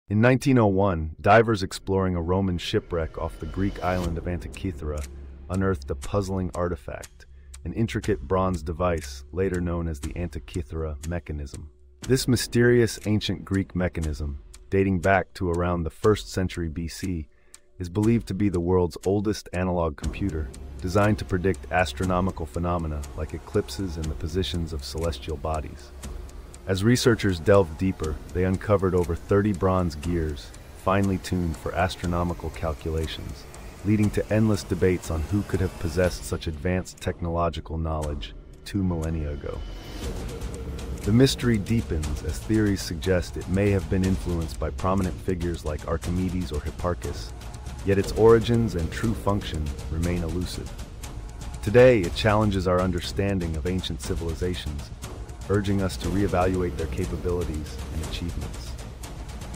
The Mystery of the Antikythera Mechanism
Unveil the enigma behind the world's oldest known analog computer, the Antikythera Mechanism, discovered in an ancient shipwreck.